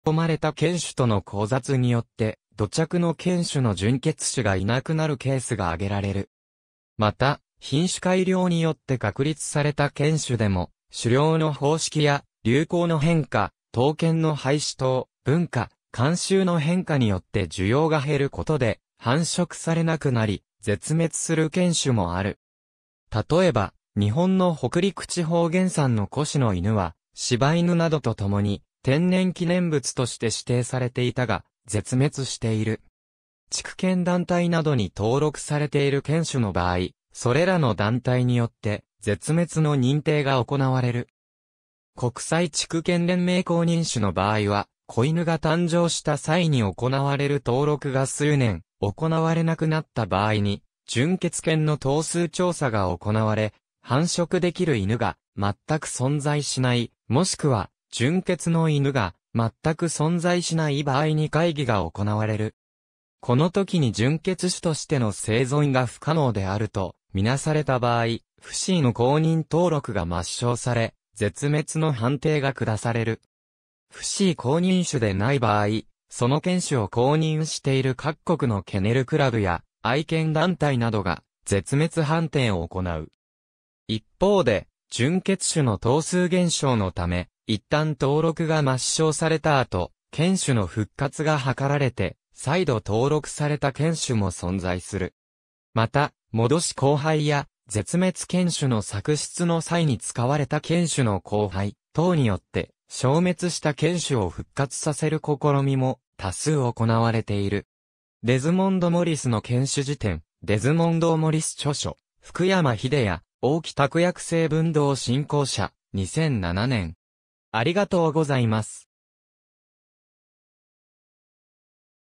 絶滅犬種
絶滅犬種（ぜつめつけんしゅ）とは、使役目的の変化や交雑などにより、純血種が絶滅した犬種である。
犬種の絶滅には、様々な要因がある。
もっとも一般的なものとして、他地域から持ち込まれた犬種との交雑によって土着の犬種の純血種がいなくなるケースがあげられる。
また、品種改良によって確立された犬種でも、狩猟の方式や流行の変化、闘犬の廃止等、文化・慣習の変化によって需要が減ることで、繁殖されなくなり、絶滅する犬種もある。
例えば、日本の北陸地方原産の越の犬は、柴犬などとともに天然記念物として指定されていたが、絶滅している。
畜犬団体（ケネルクラブ）などに登録されている犬種の場合、それらの団体によって絶滅の認定が行われる。
国際畜犬連盟（FCI）公認種の場合は、仔犬が誕生した際に行われる登録が数年行われなくなった場合に純血犬の頭数調査が行われ、繁殖できる犬が全く存在しない、若しくは純血の犬が全く存在しない場合に会議が行われる。
このときに純血種としての生存が不可能であるとみなされた場合、FCIの公認登録が抹消され、絶滅の判定が下される。
FCI公認種でない場合、その犬種を公認している各国のケネルクラブや愛犬団体などが絶滅判定を行う。
一方で、純血種の頭数減少のため、一旦登録が抹消された後、犬種の復活が図られて、再度登録された犬種も存在する。
また、戻し交配や絶滅犬種の作出の際に使われた犬種の交配等によって、消滅した犬種を復活させる試みも多数行われている（オールディ・イングリッシュ・ブルドッグ等）。
『デズモンド・モリスの犬種事典』デズモンド・モリス著書、福山英也、大木卓訳 誠文堂新光社、2007年